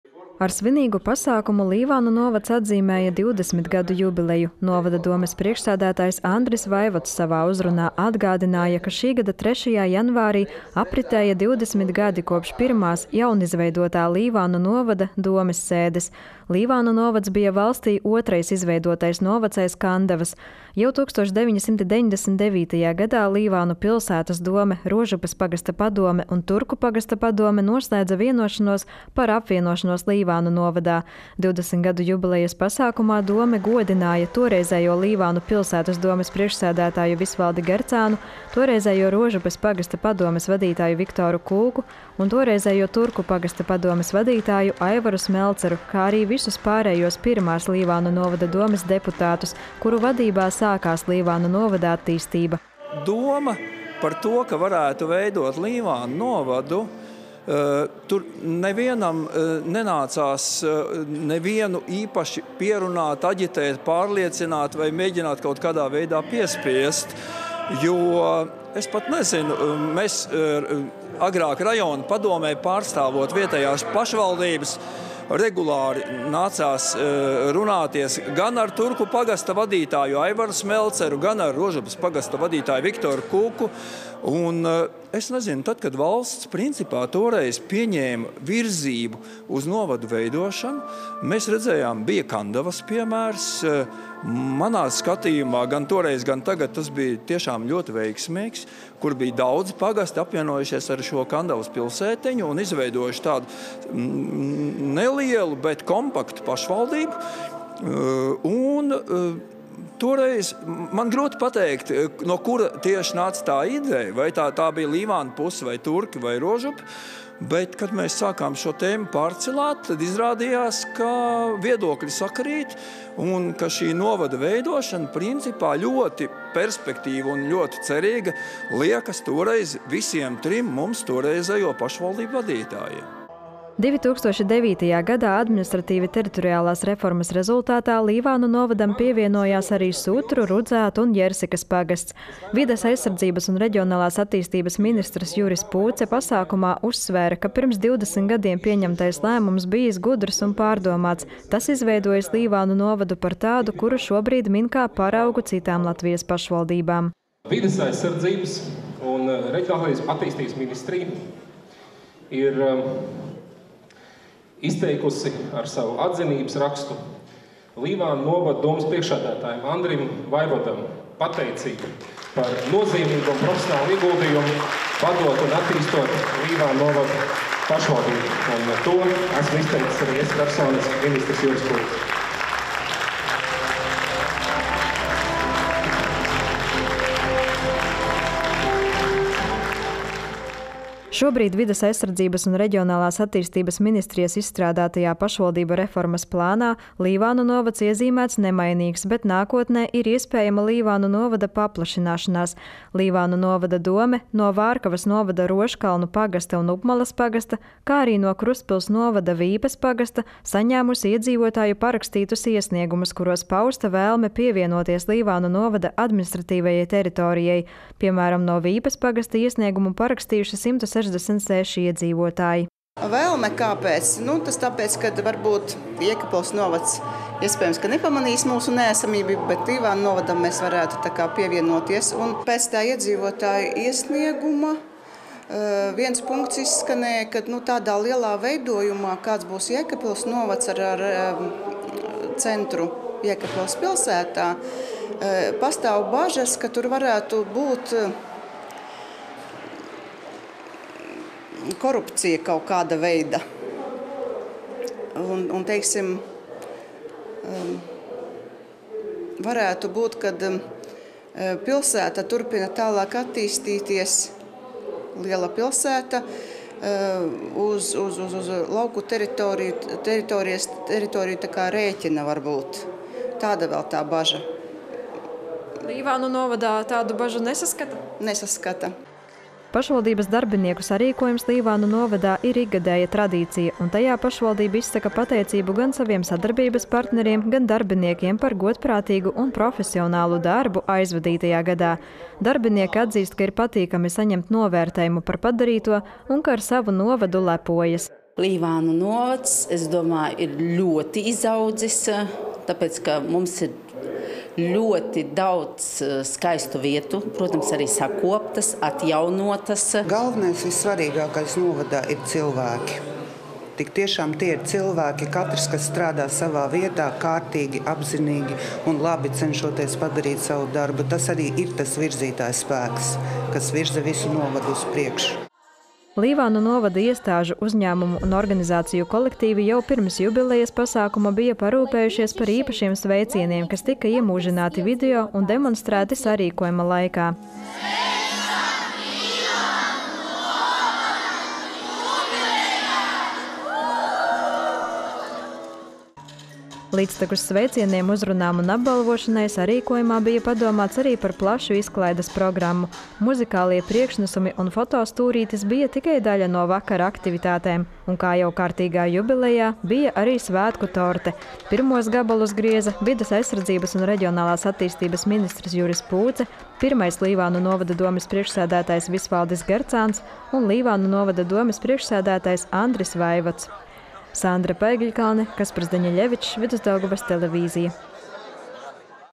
Līvānu novads svin 20 gadu jubileju
Ar svinīgu pasākumu Līvānu novads atzīmēja 20 gadu jubileju. Novada domes priekšsēdētājs Andris Vaivods savā uzrunā atgādināja, ka šī gada 3.janvārī apritēja 20 gadi kopš pirmās jaunizveidotā Līvānu novada domes sēdes. Līvānu novads bija valstī otrais izveidotais novads aiz Kandavas. Jau 1999.gadā Līvānu pilsētas dome, Rožupes pagasta padome un Turku pagasta padome noslēdza vienošanos par apvienošanos Līvānu novadā. 20 gadu jubilejas pasākumā dome godināja toreizējo Līvānu pilsētas domes priekšsēdētāju Visvaldi Gercānu, toreizējo Rožupes pagasta padomes vadītāju Viktoru Kūku un toreizējo Turku pagasta padomes vadītāju Aivaru Smelceru, kā arī visus pārējos pirmās Līvānu novada domes deputātus, kuru vadībā sākās Līvānu novada attīstība.

Viens no Līvānu novada dibinātājiem, pirmais Līvānu novada domes priekšsēdētājs Visvaldis Gercāns stāsta: “Par to, ka varētu veidot Līvānu novadu, nevienam nenācās nevienu īpaši pierunāt, aģitēt vai piespiest. Man agrāk, rajona padomē pārstāvot vietējās pašvaldības, regulāri nācās runāties gan ar Turku pagasta vadītāju Aivaru Smelceru, gan ar Rožupes pagasta vadītāju Viktoru Kūku. Kad valsts pieņēma virzību uz novadu veidošanu, mēs redzējām Kandavas piemēru. Manā skatījumā tas bija ļoti veiksmīgs piemērs, daudzi pagasti bija apvienojušies ar Kandavas pilsētiņu un izveidojuši nelielu, bet kompaktu pašvaldību. Man grūti pateikt, no kura nāca tā ideja, bet tad, kad mēs sākām šo tēmu pārcilāt, izrādījās, ka viedokļi sakrīt. Novadu veidošana mums visiem trim toreizējiem pašvaldību vadītājiem likās ļoti perspektīva, ļoti cerīga.”

2009.gadā Administratīvi teritoriālās reformas rezultātā Līvānu novadam pievienojās arī Sutru, Rudzātu un Jersikas pagasts. Vides aizsardzības un reģionālās attīstības ministrs Juris Pūce pasākumā uzsvēra, ka pirms 20 gadiem pieņemtais lēmums bijis gudrs un pārdomāts, tas izveidojis Līvānu novadu par tādu, kuru šobrīd min kā paraugu citām Latvijas pašvaldībām.

Šobrīd Vides aizsardzības un reģionālās attīstības ministrijas izstrādātajā pašvaldību reformas plānā Līvānu novads iezīmēts nemainīgs, bet nākotnē ir iespējama Līvānu novada paplašināšanās. Līvānu novada dome no Vārkavas novada Rožkalnu pagasta un Upmalas pagasta, kā arī no Krustpils novada Vīpes pagasta saņēmusi iedzīvotāju parakstītus iesniegumus, kuros pausta vēlme pievienoties Līvānu novada administratīvajai teritorijai. Piemēram, no Vīpes pagasta iesniegumu parakstījuši 166 iedzīvotāji. Vīpes pagasta pārvaldes vadītāja Ineta Šeinova saka: “Kāpēc ir šāda vēlme? Jēkabpils novads, iespējams, nepamanīs mūsu neesamību, bet Līvānu novadam mēs varētu pievienoties. Iedzīvotāju iesniegumā izskanēja bažas, ka tādā lielā veidojumā, kāds būs Jēkabpils novads ar centru Jēkabpils pilsētā, varētu būt kādi korupcijas riski un ka pilsēta varētu turpināt attīstīties uz lauku teritoriju rēķina. Līvānu novadā viņi tādas bažas nesaskata.”

Pašvaldības darbinieku sarīkojums Līvānu novadā ir ikgadēja tradīcija, un tajā pašvaldība izsaka pateicību gan saviem sadarbības partneriem, gan darbiniekiem par godprātīgu un profesionālu darbu aizvadītajā gadā. Darbinieki atzīst, ka ir patīkami saņemt novērtējumu par padarīto un ka ar savu novadu lepojas. Līvānu 1.vidusskolas Laimiņas skolas ēkas dežurante Ināra Boļšakova saka: “Es domāju, ka Līvānu novads ir ļoti izaudzis, jo mums ir ļoti daudz skaistu vietu, kuras ir sakoptas un atjaunotas.”

SIA “Līvānu dzīvokļu un komunālā saimniecība” kasiere, noliktavas pārzine Irēna Briede saka: “Galvenais un svarīgākais Līvānu novadā ir cilvēki, tik tiešām tie ir cilvēki – katrs, kas strādā savā vietā kārtīgi un apzinīgi, cenšoties labi padarīt savu darbu. Tas arī ir tas virzītājspēks, kas virza visu novadu uz priekšu.”

Līvānu novada iestāžu, uzņēmumu un organizāciju kolektīvi jau pirms jubilejas pasākuma bija parūpējušies par īpašiem sveicieniem, kas tika iemūžināti video un demonstrēti sarīkojuma laikā.

Līdztekus sveicieniem, uzrunām un apbalvošanai sarīkojumā bija padomāts arī par plašu izklaides programmu. Muzikālie priekšnesumi un foto stūrītis bija tikai daļa no vakara aktivitātēm. Un, kā jau kārtīgā jubilejā, bija arī svētku torte. Pirmos gabalus grieza vides aizsardzības un reģionālās attīstības ministrs Juris Pūce, Līvānu novada domes pirmais priekšsēdētājs Visvaldis Gercāns un Līvānu novada domes priekšsēdētājs Andris Vaivods.

Sandra Paegļkalne